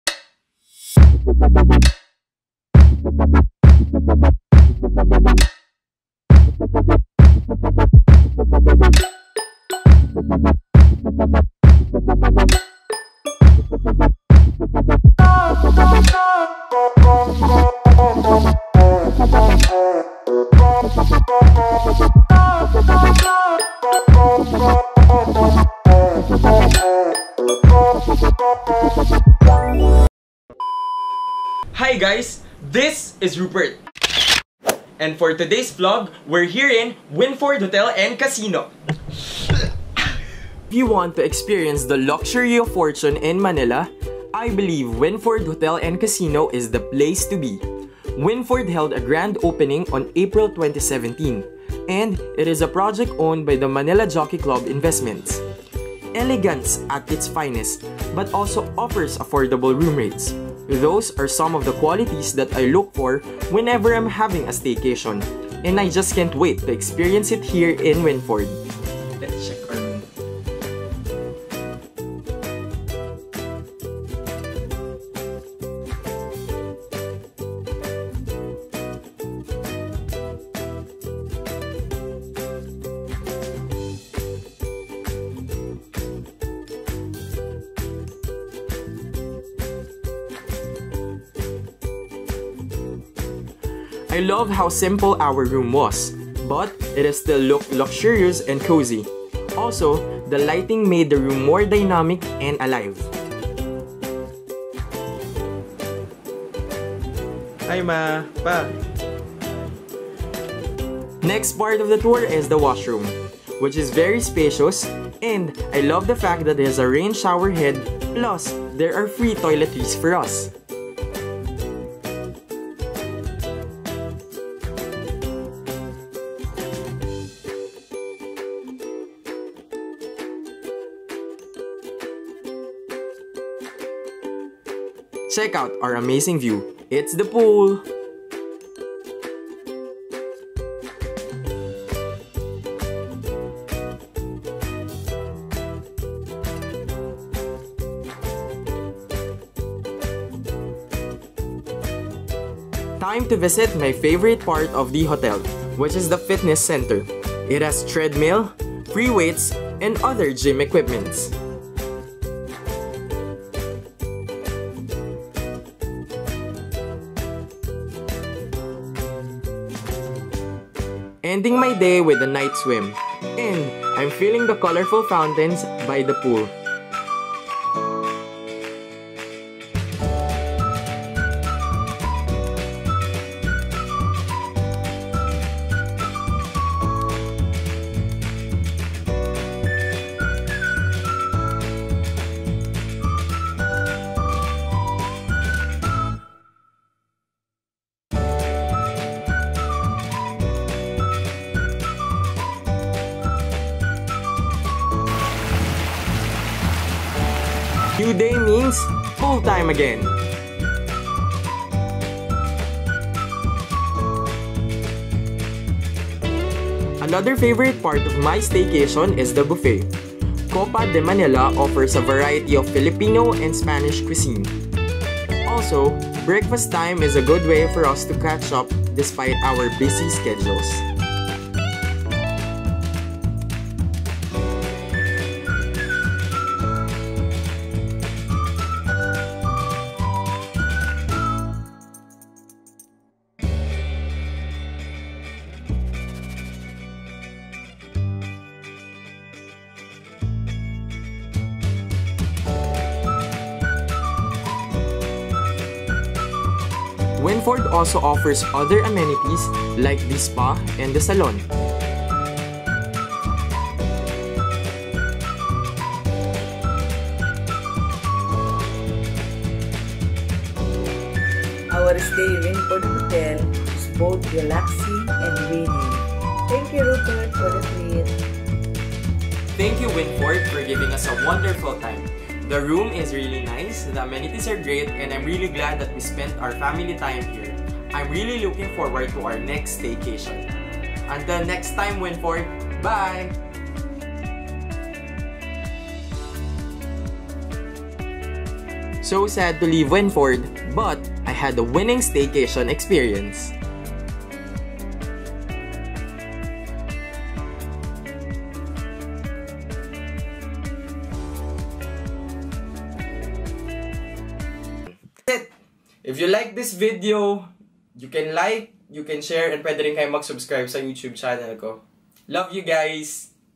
Vlog 17: WINFORD HOTEL STAYCATION EXPERIENCE (SULIT BA!?) | RUPERT PARAS VLOGS
WATCH IN HD - Vlog 17 : IF YOU WANT TO EXPERIENCE THE LUXURY OF FORTUNE IN MANILA! I BELIEVE WINFORD HOTEL IS THE PLACE TO BE! WATCH MY NEW VLOG TO SEE IT FOR YOURSELF!
Winford Manila Resort and Casino